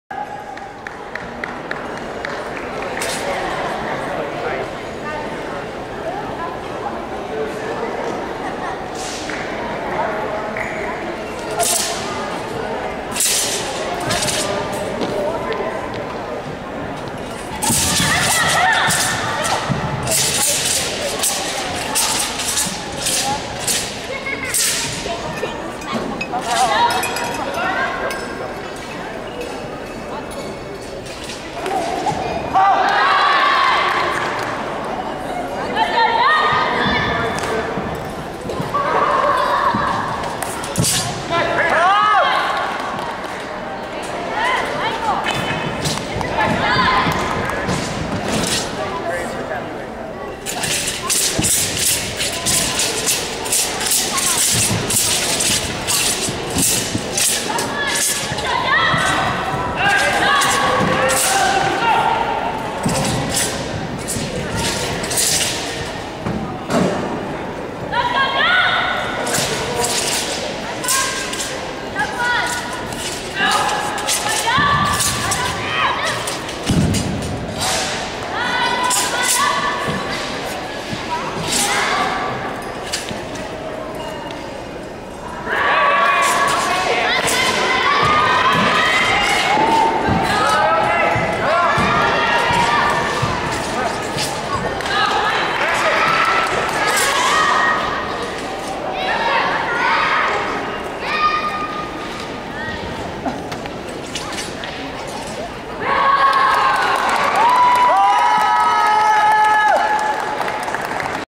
2017 USAWKF Team Trials and Nationals - Dominic Chow - Broadsword
O-mei students participated at the 2017 USAWKF Team Trials and Nationals
Civic Center Exhibit Hall - Lubbock, Texas